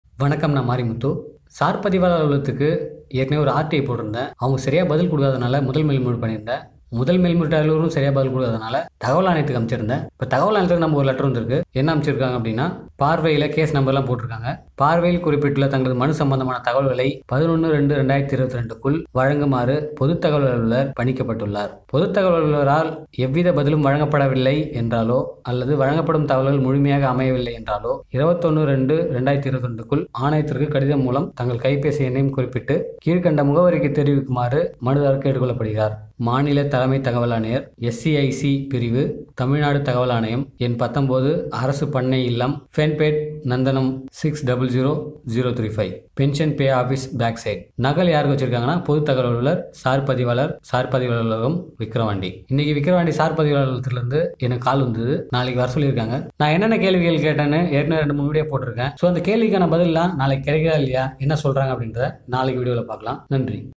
TN information commission reply for RTI
telling the reply of TN information commission about Register office.